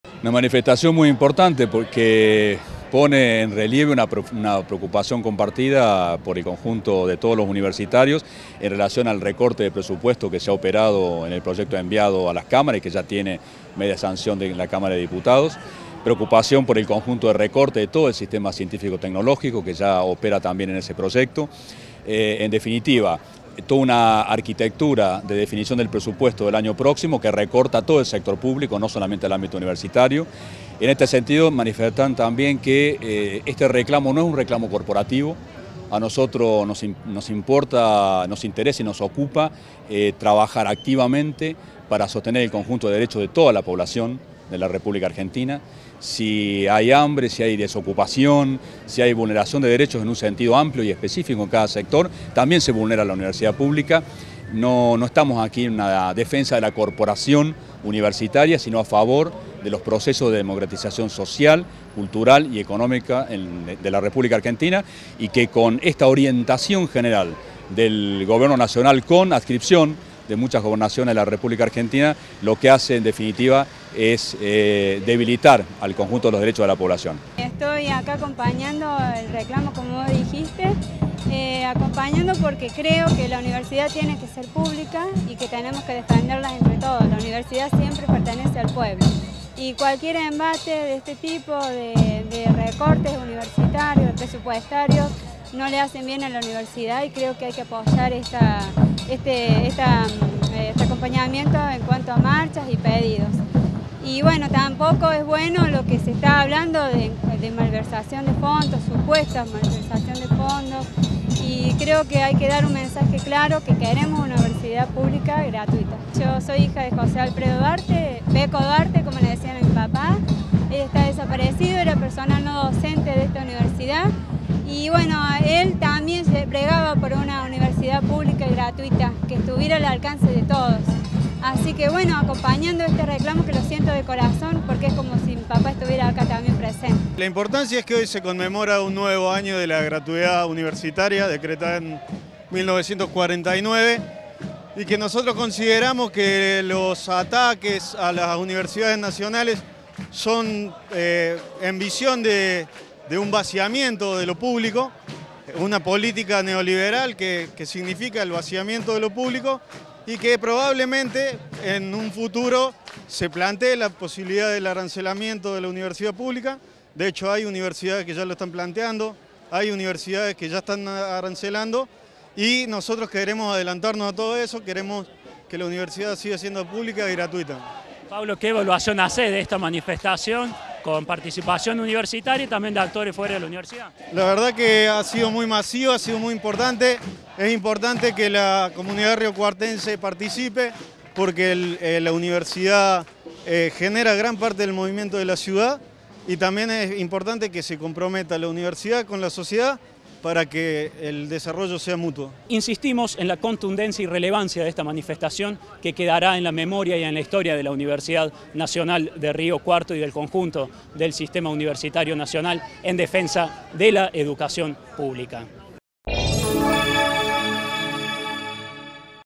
MARCHA POR LA EDUCACION PUBLICA - MARCELO RUIZ, CLARISA DUARTE Y PAULO ZAMBRONI
MARCELO RUIZ: “ESTE NO ES UN RECLAMO CORPORATIVO”
CLARISA DUARTE: “ES COMO SI MI PAPA ESTUVIESE ACA PRESENTE”
PAULO ZAMBRONI: “QUEREMOS QUE LA UNIVERSIDAD SIGA SIENDO PUBLICA Y GRATUITA”

Facebook.com/uniriotv
Twitter.com/uniriotv
Instagram.com/uniriotv
Web - uniriotv.unrc.edu.ar
o envianos un email a
UniRio TV
Universidad Nacional de Río Cuarto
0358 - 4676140